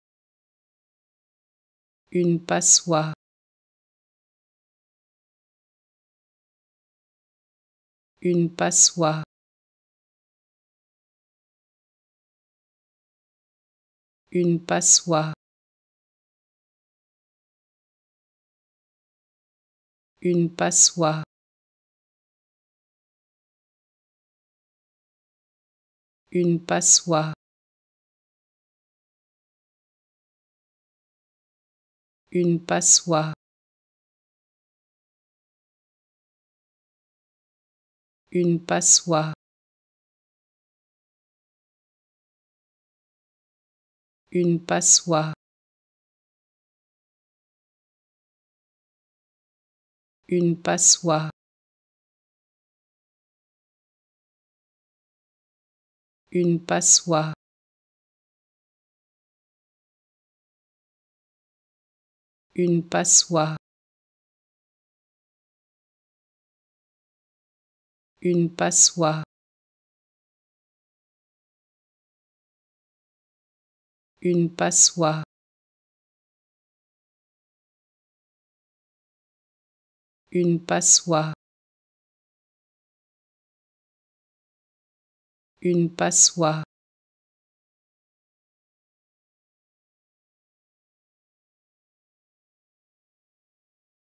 How to Pronounce ''Une passoire'' (A colander) Correctly in French
Learn how to say and properly pronounce ''Une passoire'' in French with this free pronunciation tutorial.